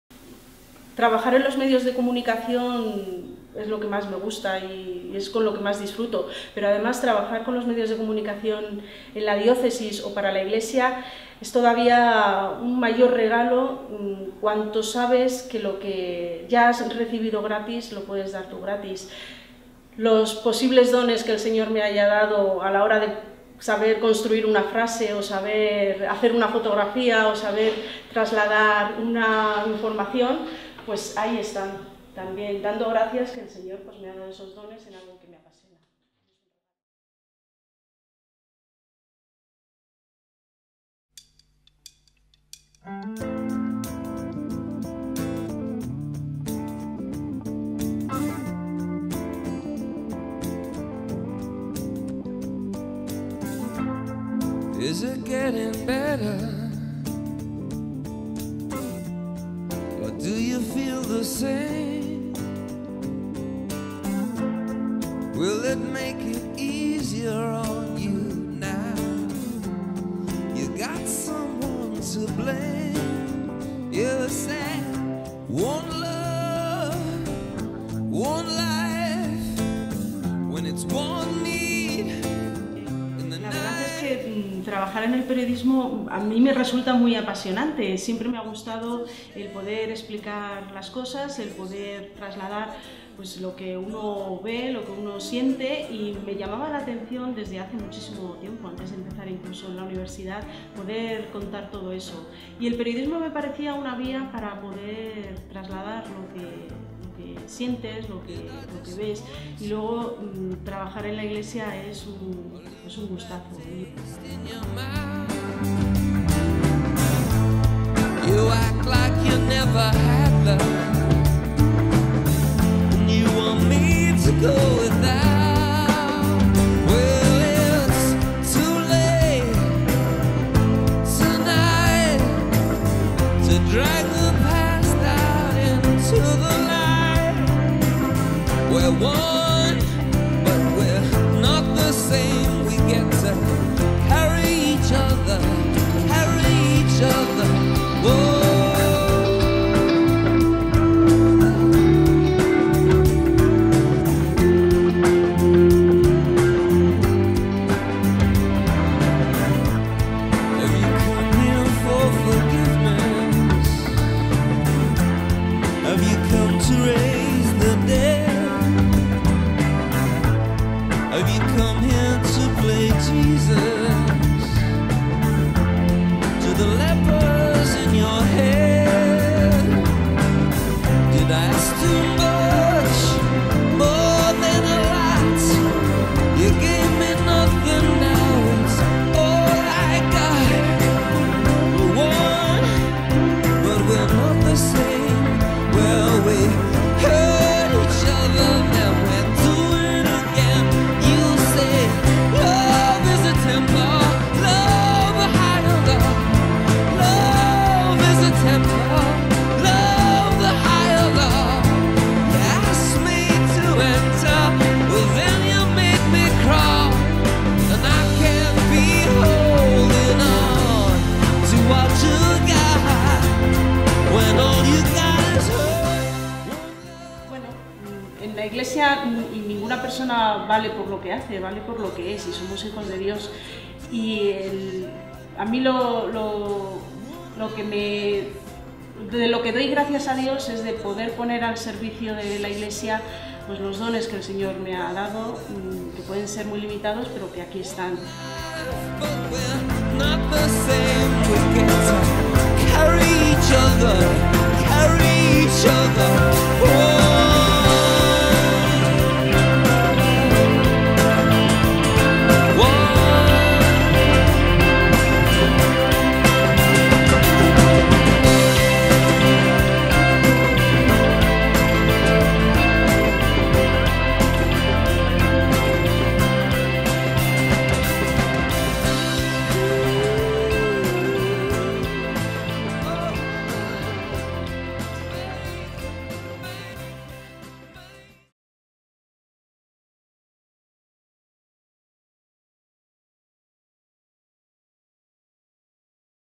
II Premio de Comunicación San Francisco de Sales (Granada)
Vídeo del Secretariado de Medios de Comunicación Social del Arzobispado de Granada, emitido el 9 de marzo de 2018, en el acto de entrega del II Premio de Comunicación San Francisco de Sales, que otorga esta parroquia de Granada, a la periodista y Directora del Secretariado de Medios de Comunicación Social del Arzobispado, Paqui Pallarés, en reconocimiento a su trayectoria profesional como Directora de dicho Secretariado de Medios, del que forma parte desde hace 11 años.